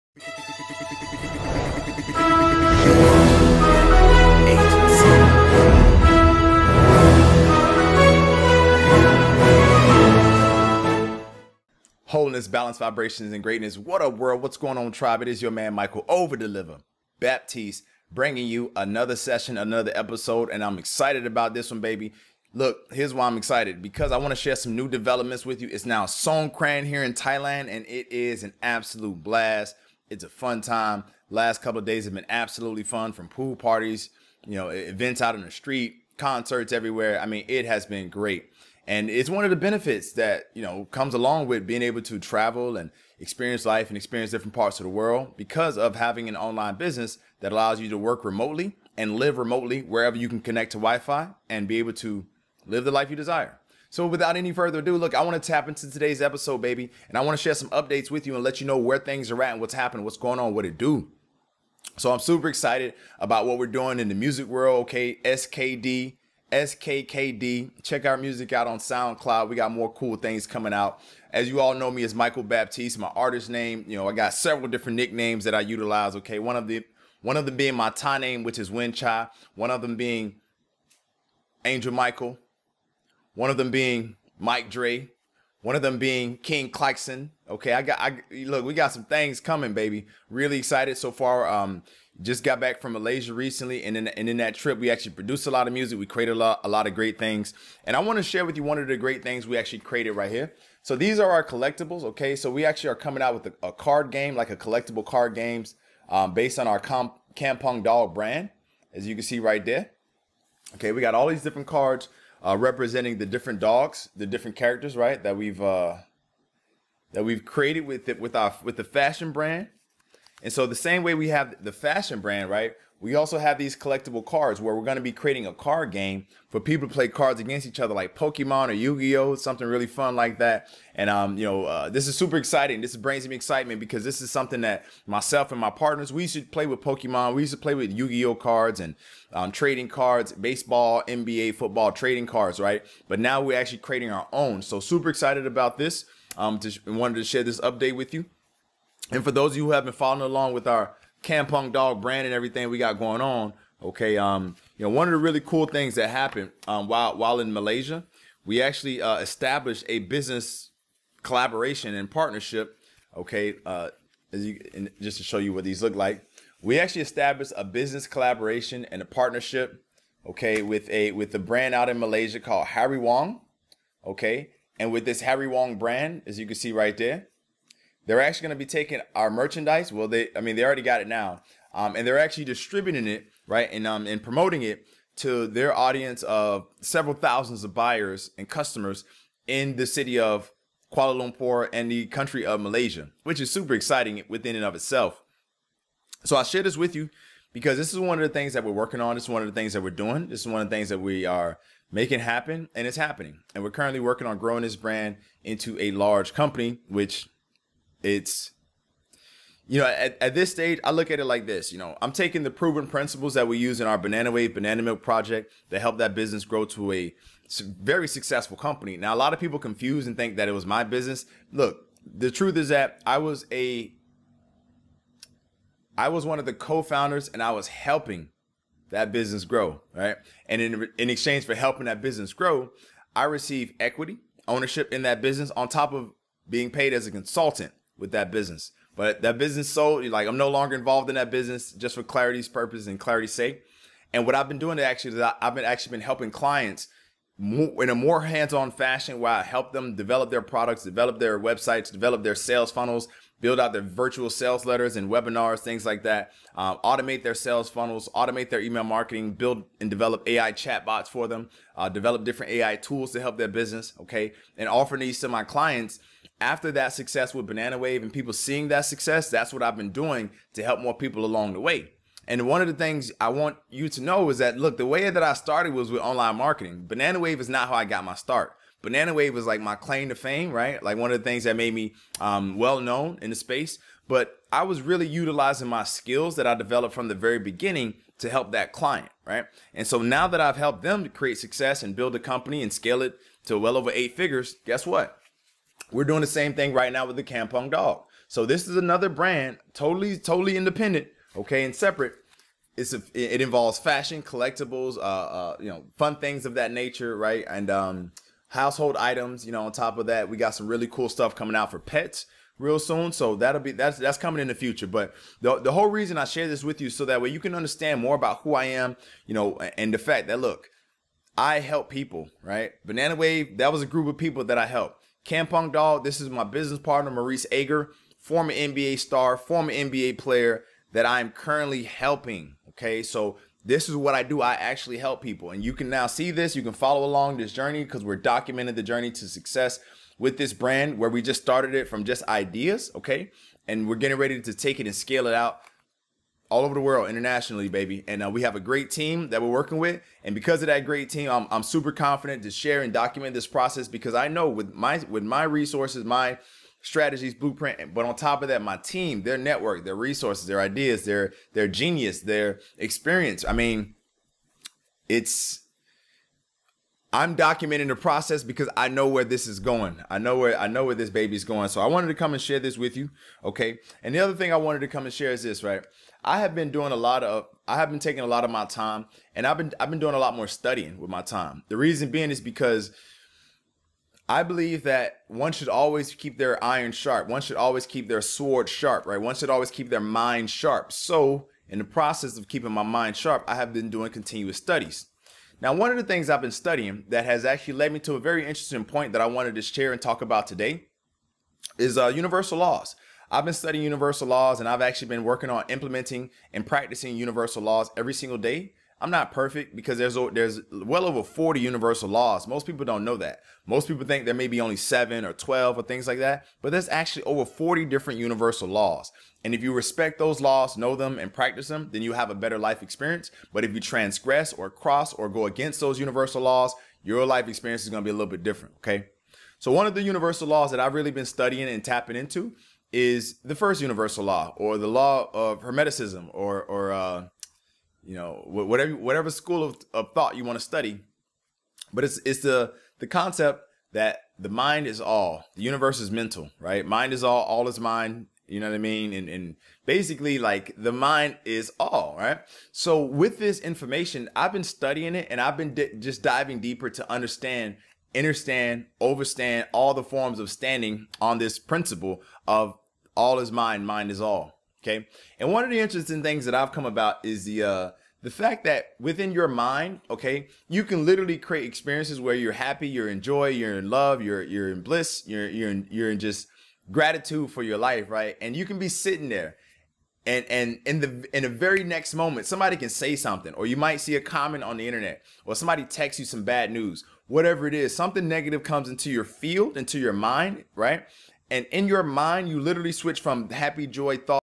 Is The Mind All? Learn Is The Mind All And How To Use This Universal Law To Achieve Greater
Did you know that the mind is all?

This is a concept that might not make total sense to you up front, but if you dive into this episode then you'll learn first hand why the mind is all.

In fact, this is no secret at all...

...it's a powerful universal law that exists within and without.

Dive deep into this episode where we tap in and discuss the power of The Mind is All, and how to use your mind to achieve greater results in your life and business.